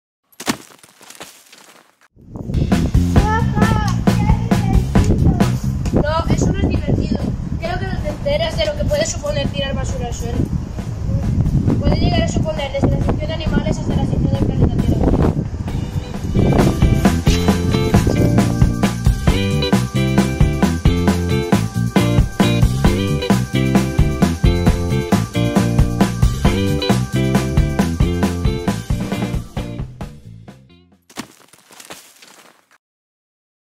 ¡Esto no es divertido!
Un niño enseña a otro a tener actitud crítica frente al cuidado del medio ambiente

IES DE VILALONGA 1º ESO C/ Rede Solidaria- Lola Rea e Candela Pérez